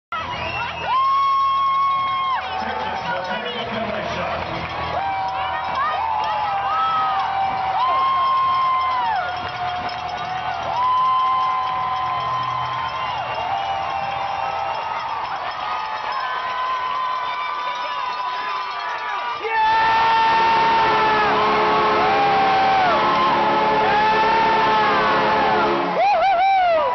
Mike Hedden penalty shot
Texas Stars F Mike Hedden converts on a 2nd period penalty shot against the Oklahoma City Barons on 14 April 2012.